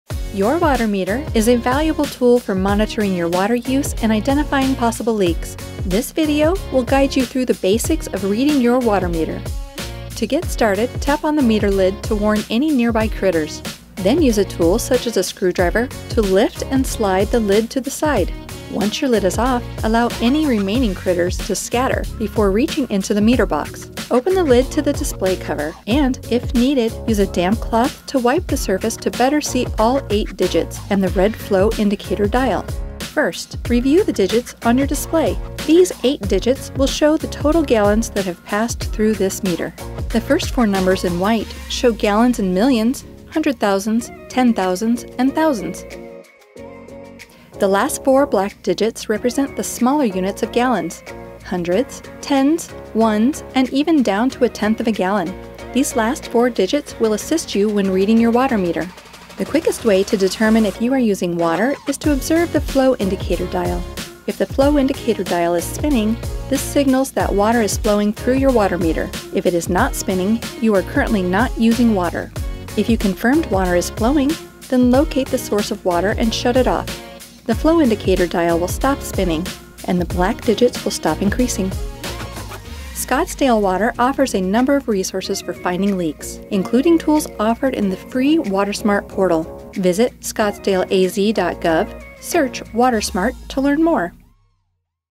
How To Read Your Water Meter | Scottsdale Water
Understanding your water meter is essential for tracking water usage, detecting leaks, and managing your water consumption effectively. In this quick tutorial, learn how to read your Scottsdale Water Meter.

Take advantage of the free resources offered by Scottsdale Water to help you locate your water leaks.

Visit ScottsdaleAZ.gov and search “WaterSmart” to learn more.